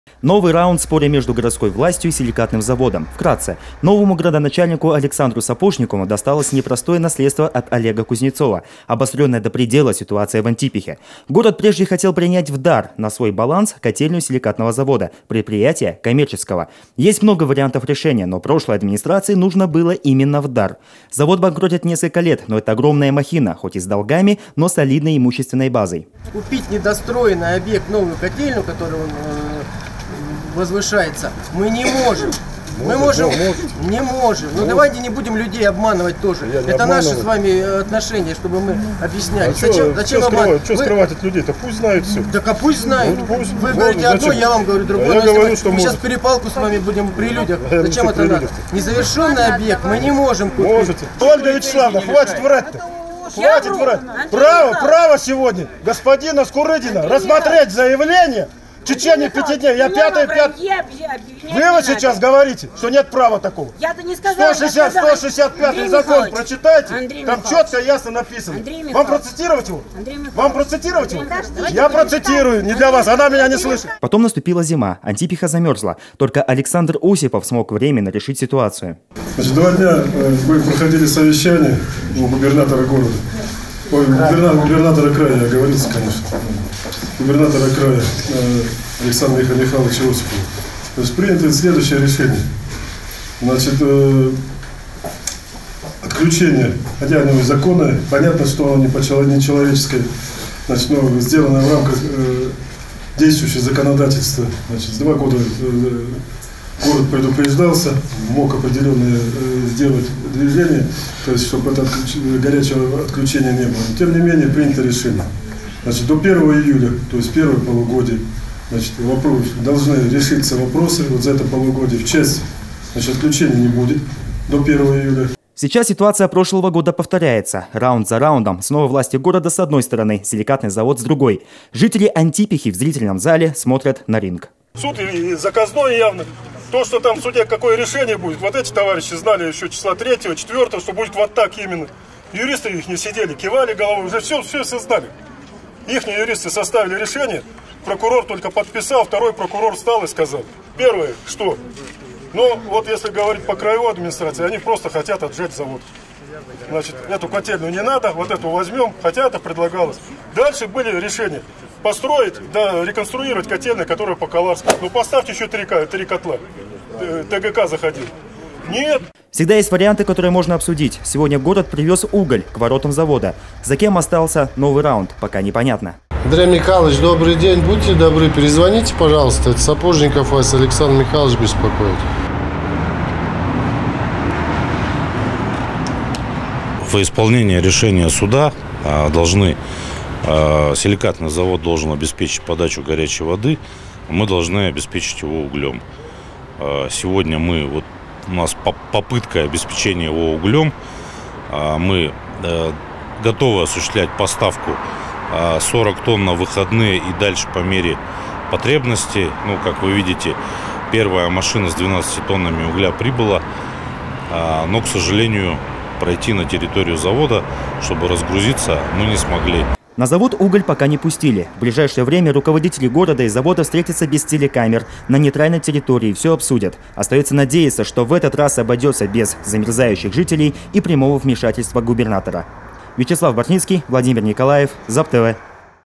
Новый раунд: Город Чита/Силикатный завод
Жители Антипихи живут без горячей воды с 4 июня – Силикатный завод остановил работу котельной из-за убытков, о чём предупреждал городские власти. Спустя месяц жители заявили о готовности объявить голодовку, если проблема не будет оперативно решена.